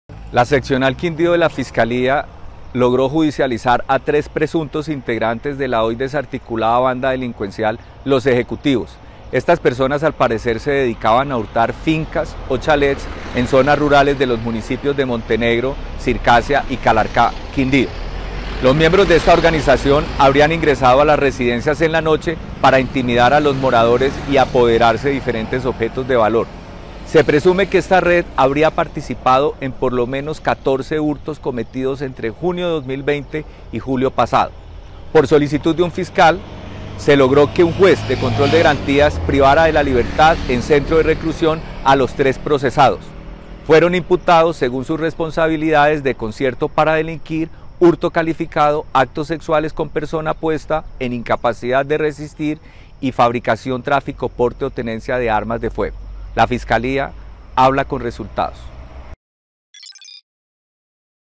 Desarticulan a ‘Los Ejecutivos’, banda dedicada a hurtar chalets en zona rural de Quindío
Ante un juez de Montenegro, Quindío, fueron presentados tres hombres por presuntamente hacer parte de la organización delincuencial ‘Los Ejecutivos’, que seleccionaría chalets o fincas para cometer en ellas hurtos mediante la modalidad de ventosa o utilizando la violencia.

Los implicados ingresarían en la noche a los domicilios y tras intimidar a los residentes los habrían despojado de sus pertenencias.